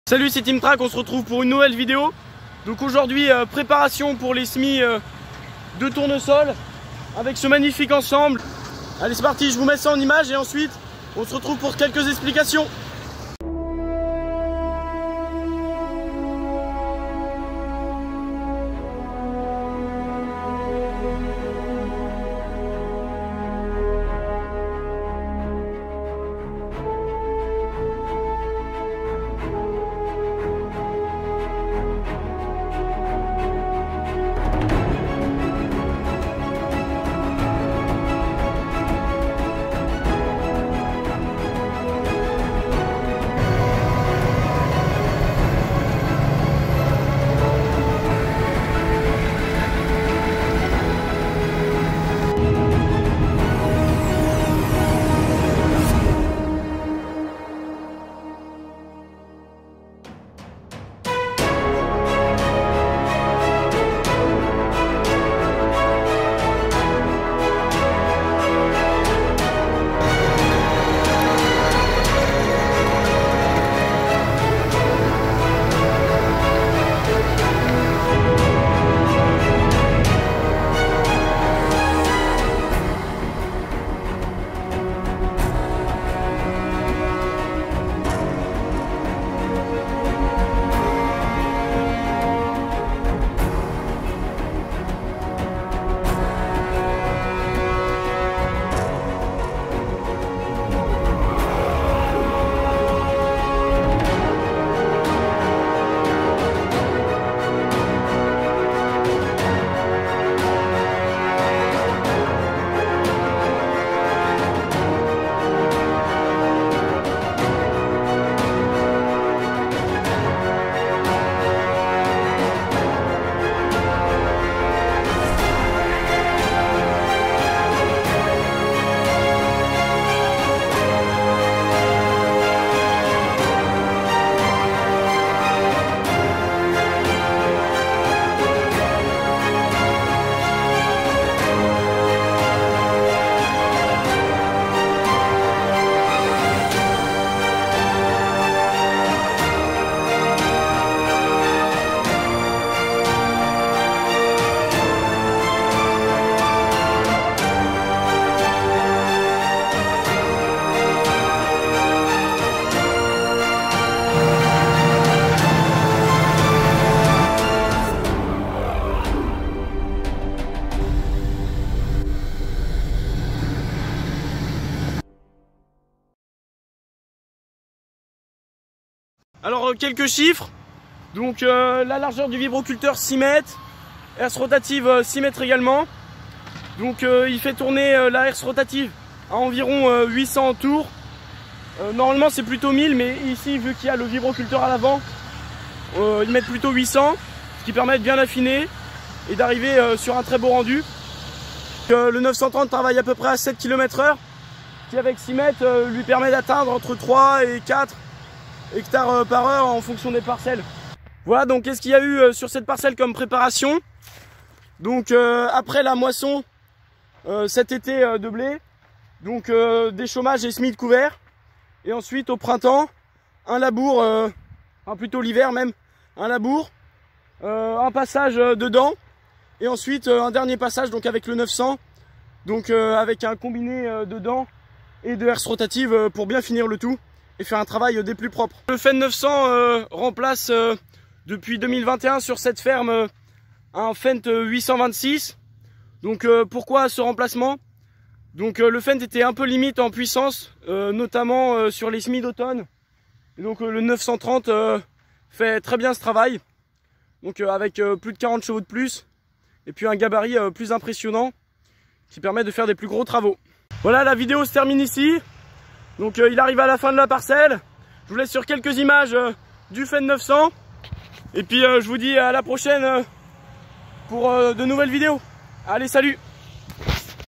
Un Vibro fait maison sur le Fendt 930 pour les préparations de Tournesols 🌻.
Salut, c'est Timtrac

   On retrouve aujourd'hui pour la reprise des travaux au printemps, et plus particulièrement pour la préparation des semis tournesol🌻 avec ce magnifique Fendt 930🚜.
 Notez qu'il est équipé d'un vibroculteur 6 mètres fait maison qui il fait sa première campagne.
   J'espère que ça vous aura plu.

                              À très bientôt pour le semis🙋‍♂️.